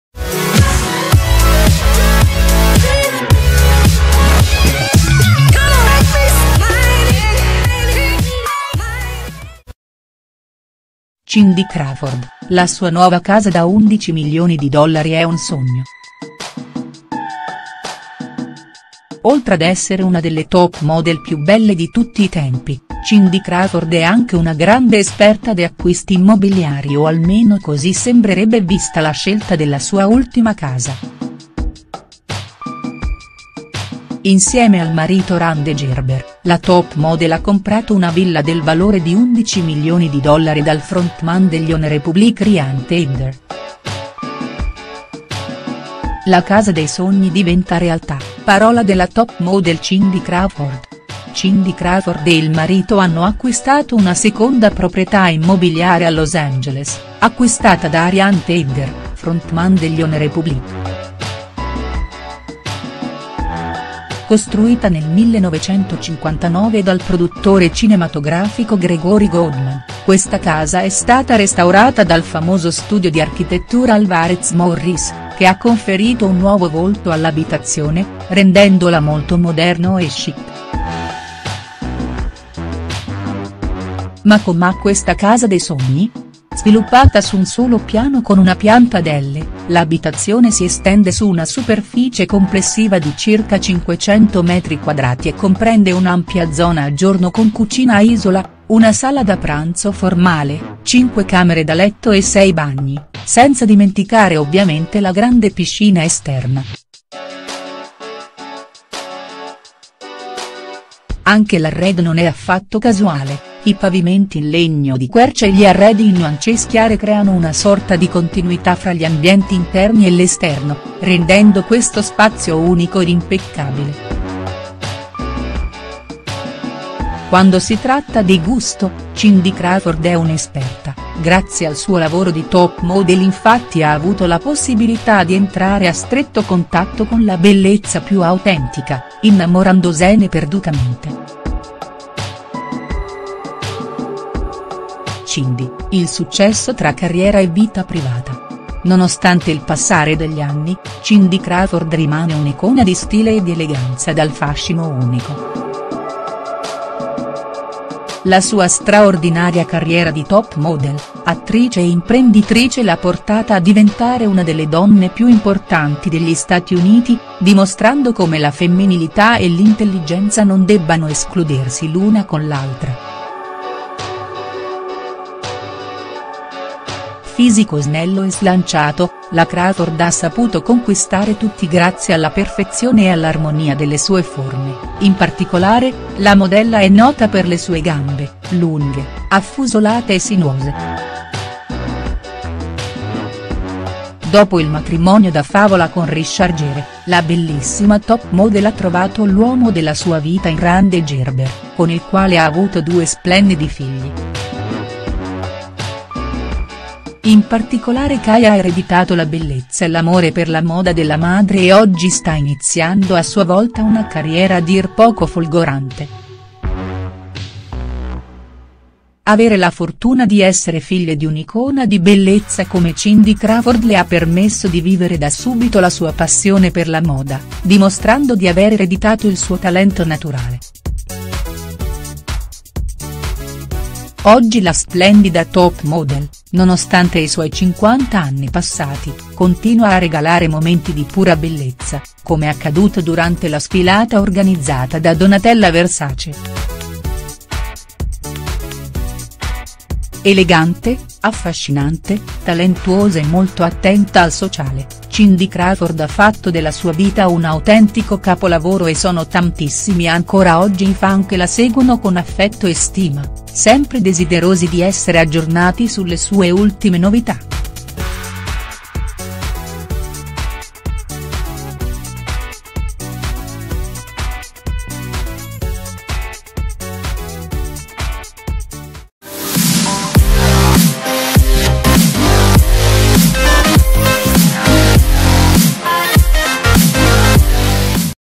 Cindy Crawford: la sua nuova casa da 11 milioni di dollari è un sogno.HD
Bellissima e dal fascino senza età, Cindy Crawford ha coronato un altro grande sogno: ha acquistato una casa meravigliosa del valore di ben 11 milioni di dollari a Beverly Hills. Una villa con piscina che lascerebbe senza fiato anche il più esigente degli acquirenti: Cindy non sbaglia un colpo.